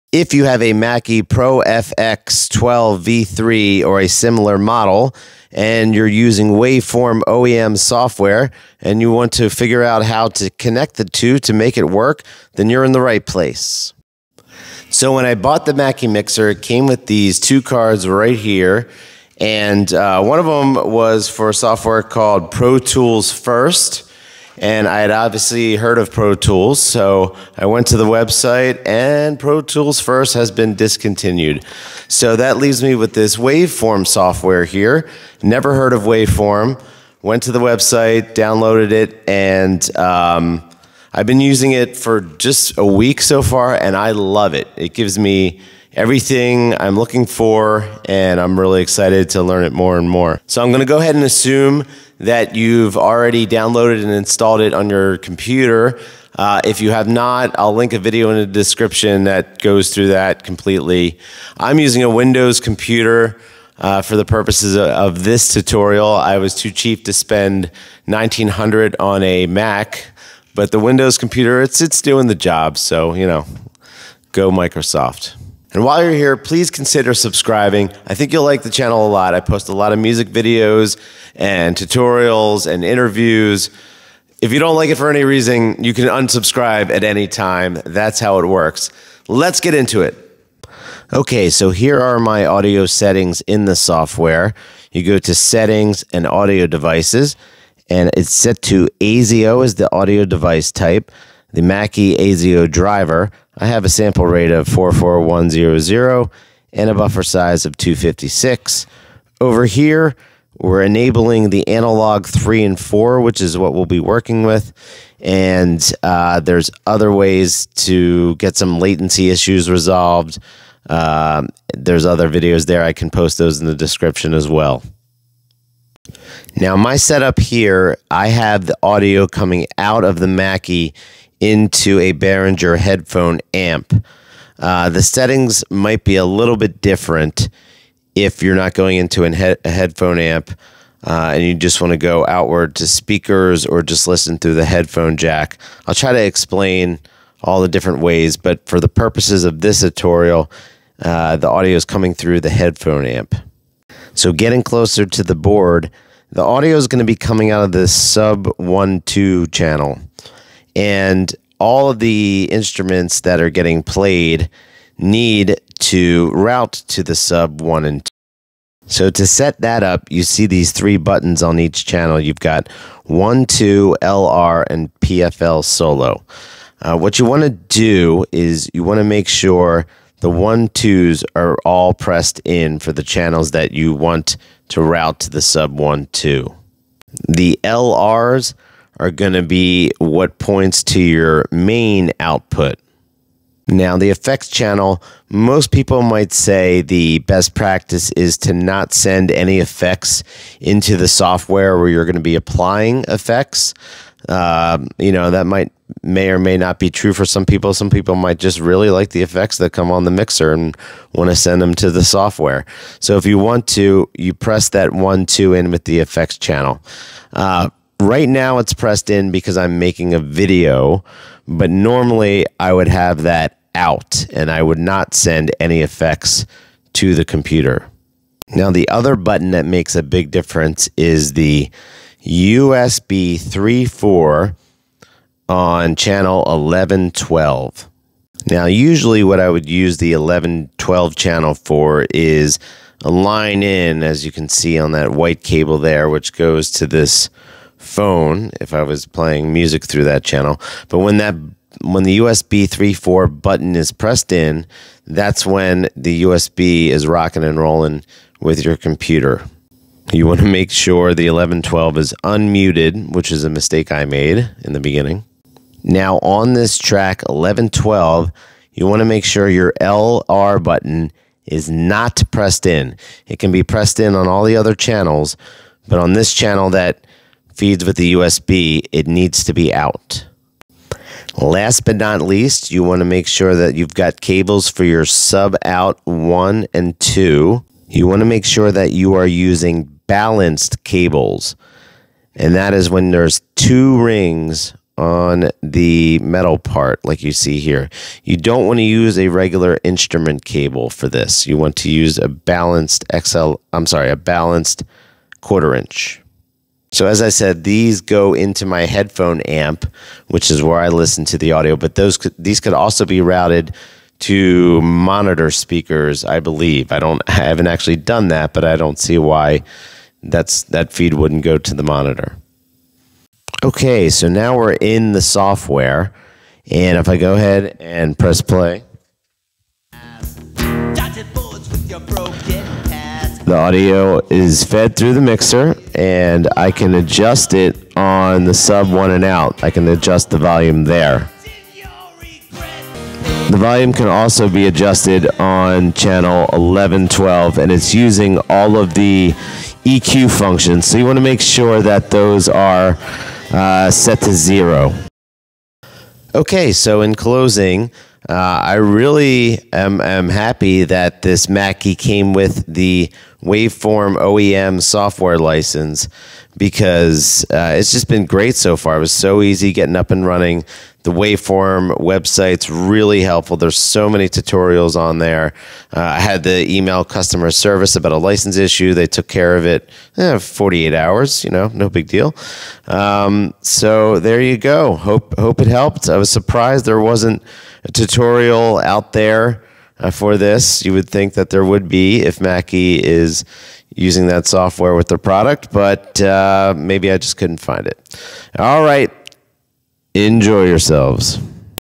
Mackie ProFX12v3 to Waveform OEM set up tutorial
How to configure your Mackie mixer to record and mix music in Waveform OEM.  Below are additional links mentioned in the video:

(scroll way down to Specs & Downloads)

Balanced cables